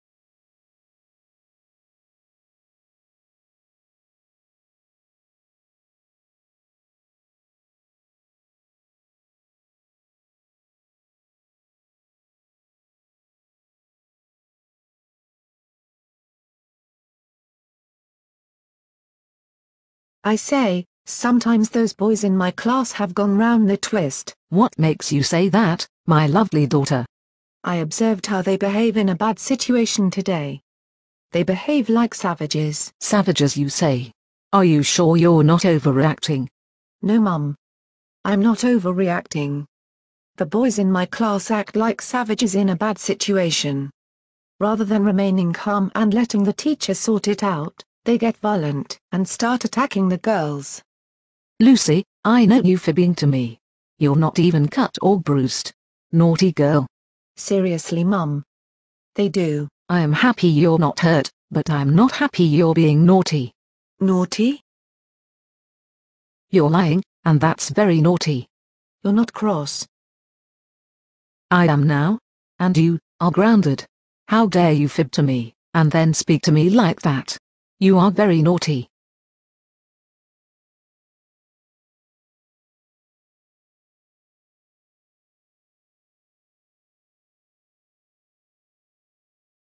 The Lucy Morgan Project (S04E07: Round the Twist)
Lucy is grounded for not only lying about savagely behaved boys, but speaking rudely to Emma as well